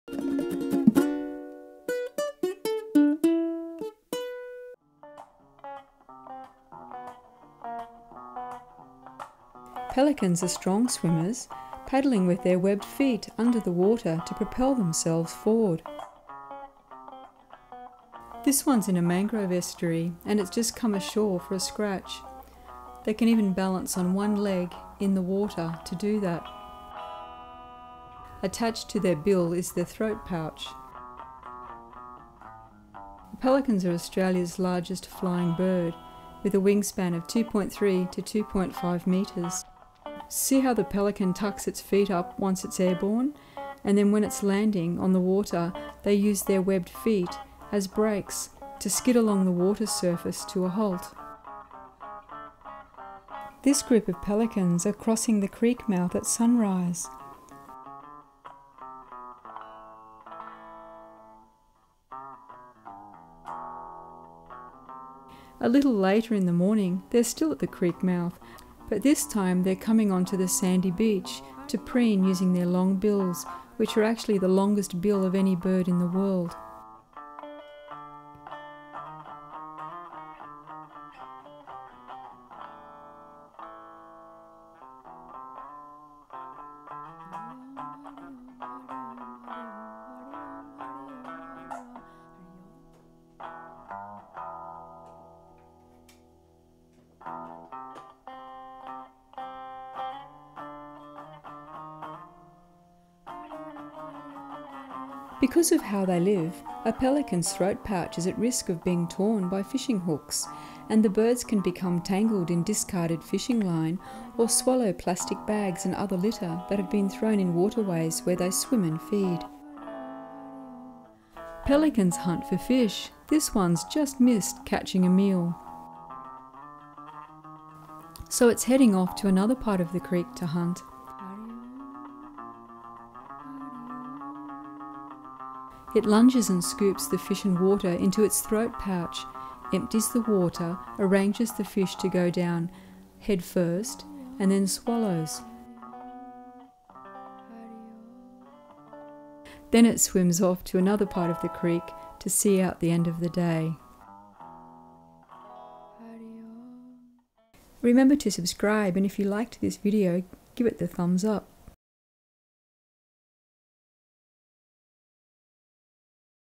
Pelicans - Austraila's largest flying bird
Australia's largest flying bird. Usually seen roosting on sand banks and beaches especially around tidal creeks or mangrove estuaries and bays. Normally feeds alone, will also work cooperatively with other pelicans to herd fish into shallow water. Filmed with bmpcc and bmpcc 4k.